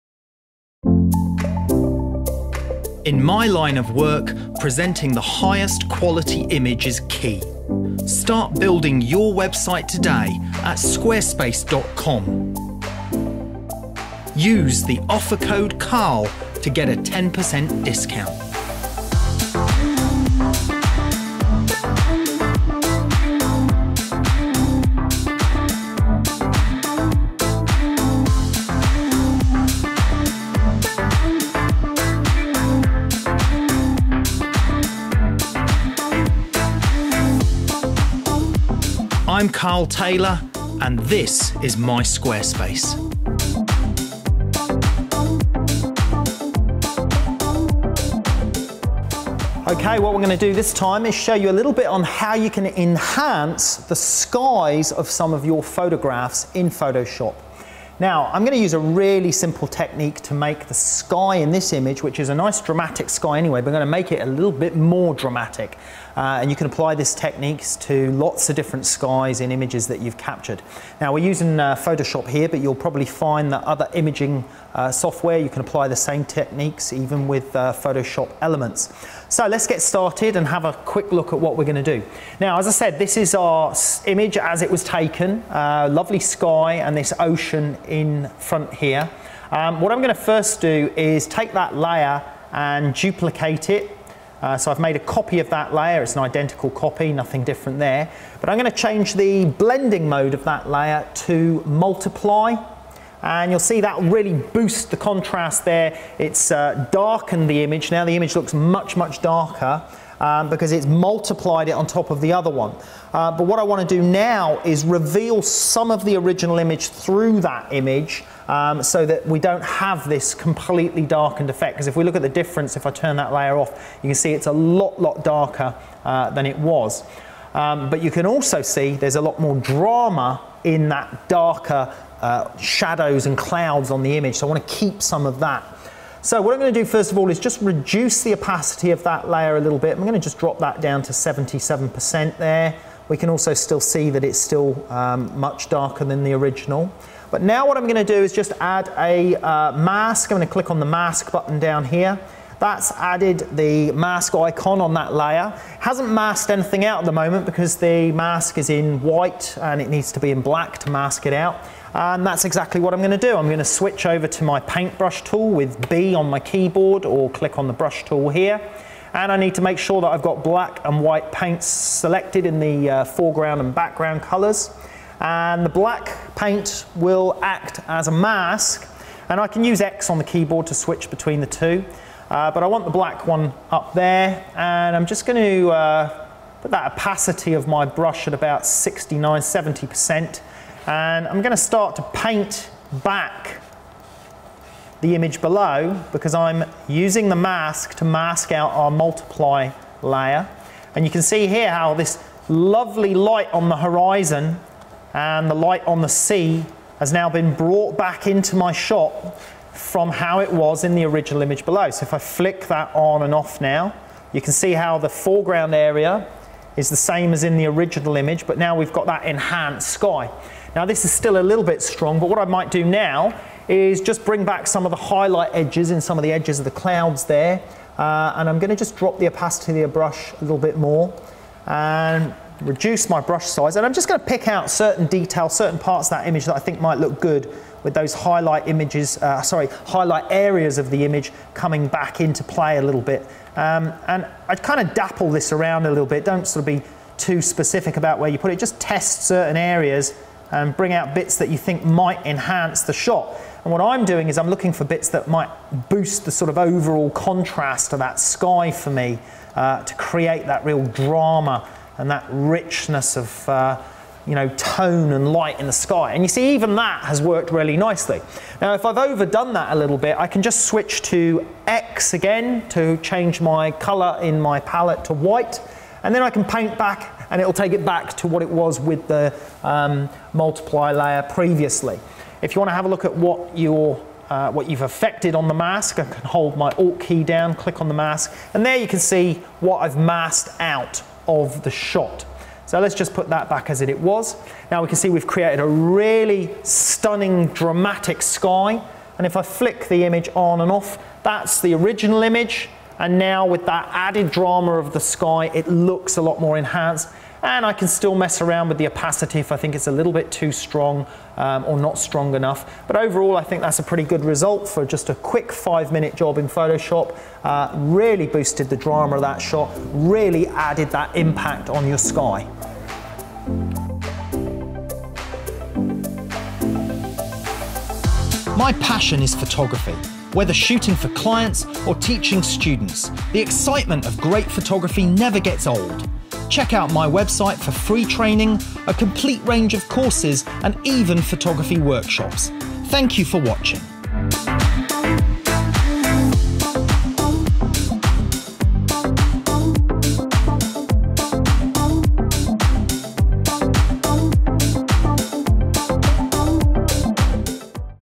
Make your Sky Outstanding with this Top Photographer Trick!
Make your Sky Outstanding with this Top Photographer Trick! - What I'm going to show you today is how you can enhance the sky in some of your photos. So if you've ever taken a shot but feel the sky needs a lift then this knowledge is for you.

I'm using Photoshop and you can apply this technique to any of your photos. You'll probably find that other photo editing software enables you to use the same techniques.

Karl

SAVE 50% on ALL our DVD courses while they're in stock